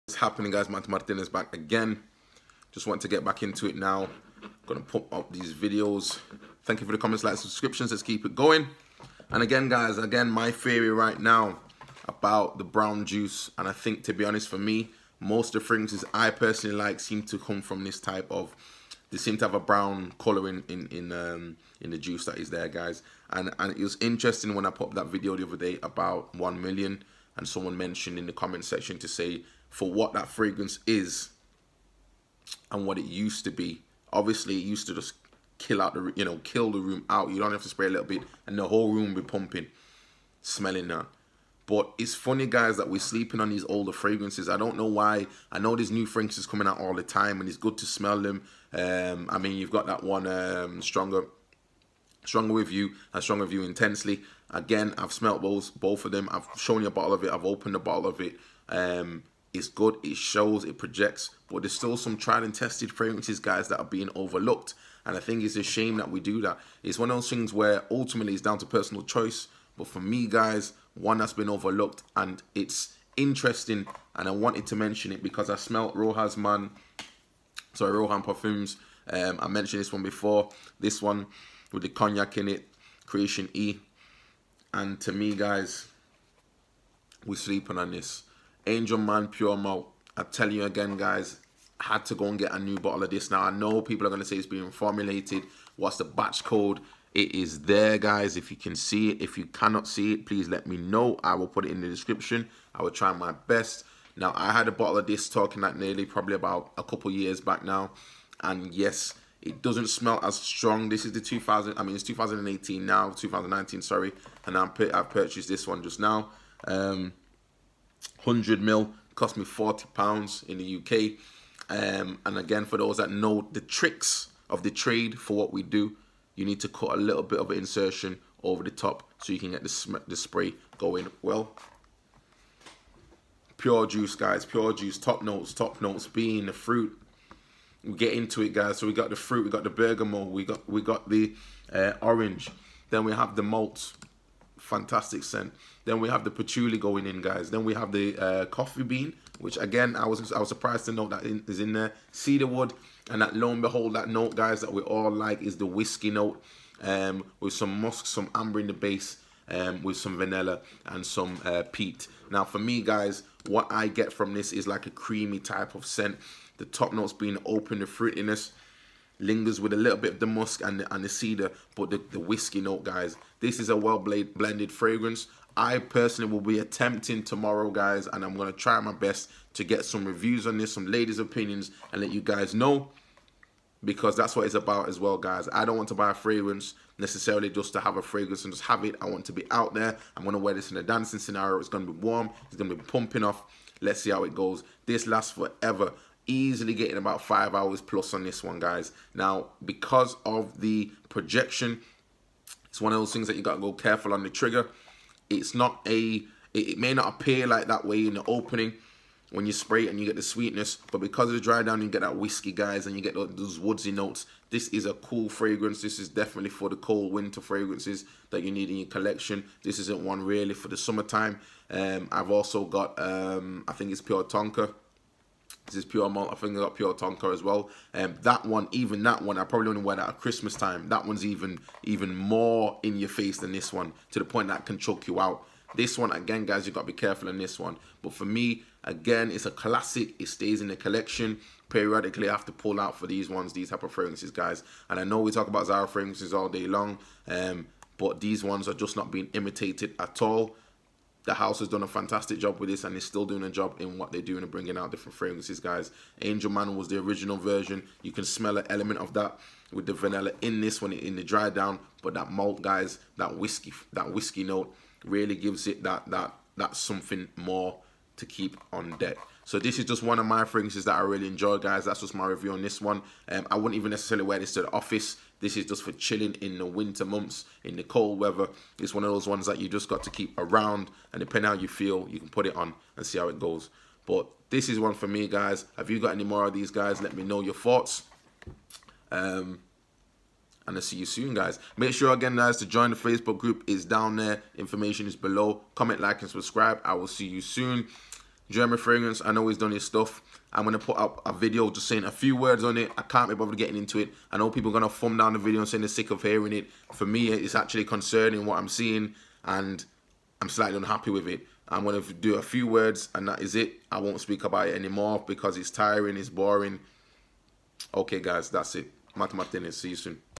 Don’t sleep on the Old Skool Mens Fragrance Review 😃✨ Pure Malt - Mugler
Any amounts donated are much appreciated and will go towards to purchase of new fragrances, content and equipment. Even if its £1 🙂